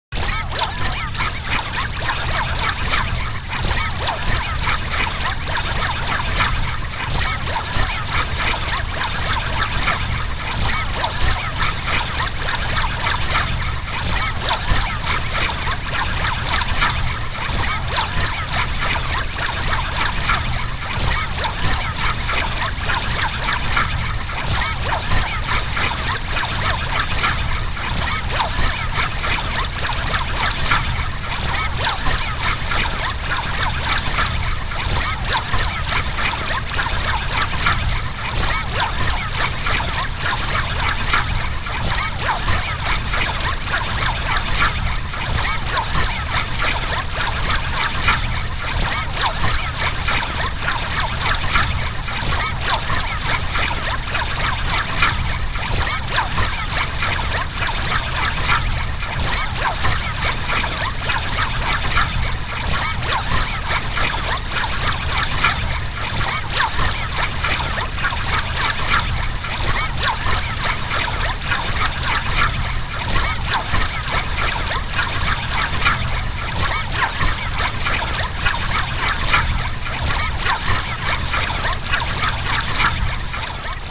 Robotic chatter - Sound effects
The sound of robots talking between themselves. If you use this sound, please link back to this video.